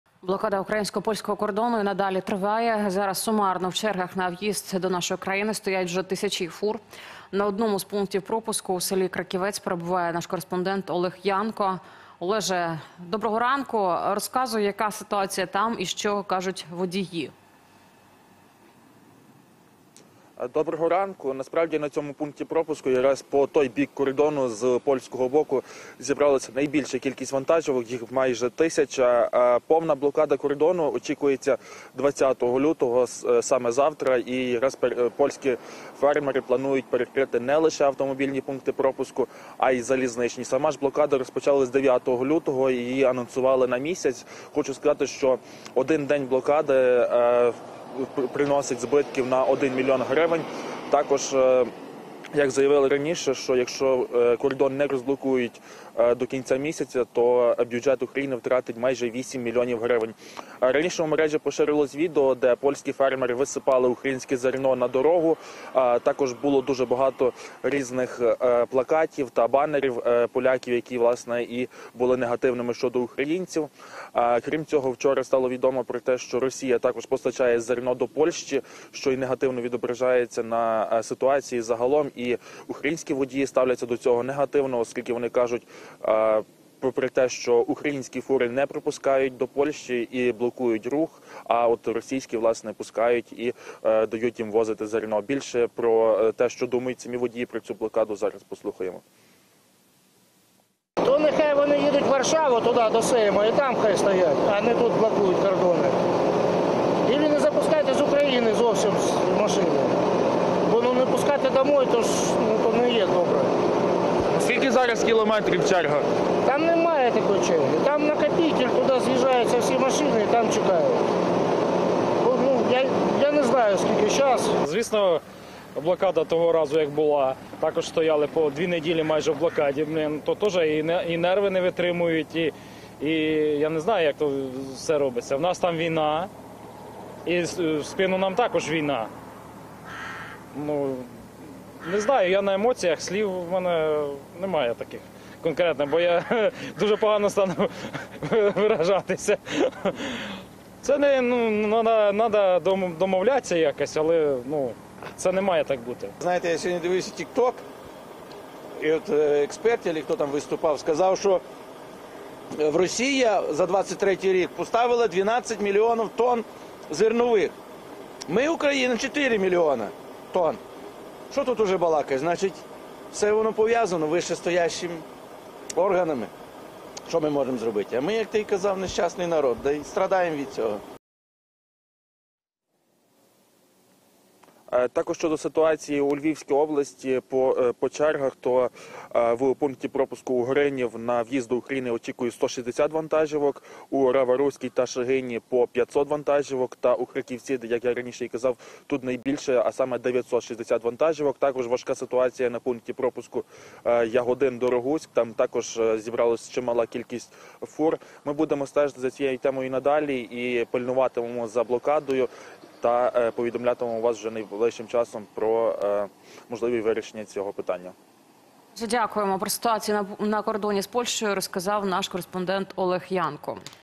ЖЕСТЬ! МАСШТАБНА блокада кордону: поляки ПЕРЕКРИЮТЬ ще й ЗАЛІЗНИЦЮ?
Блокада українсько-польського кордону й надалі триває. Зараз в черзі стоять тисячі фур. Яка нині ситуація на кордоні? Та що кажуть водії? Детальніше в сюжеті.